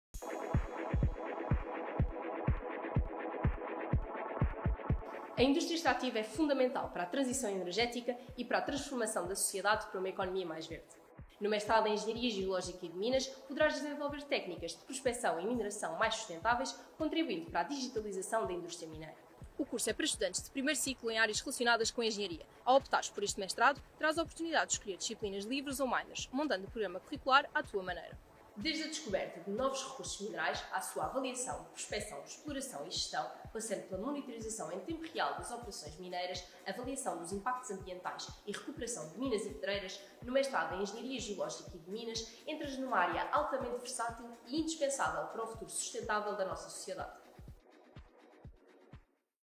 Mestrado em Engenharia Geológica e de Minas no Técnico Lisboa
Os programas de Mestrado oferecidos pelo Instituto Superior Técnico inserem-te num mundo de inovação e especialização em diversas áreas da ciência, tecnologia e engenharia.

A indústria extrativa assegura a exploração e gestão sustentável das matérias-primas, essenciais para a transição energética e transformação da nossa sociedade para uma economia mais verde. A Engenharia Geológica e de Minas desenvolve técnicas de prospeção e mineração, promovendo a digitalização da indústria mineira, assente nas melhores práticas de sustentabilidade ambiental, segurança, smart mining, automação e robótica. O papel de um Engenheiro de Minas passa pela exploração, valorização e gestão mineira, mas também pelo apoio a obras de construção, nomeadamente túneis e outras escavações em maciços rochosos.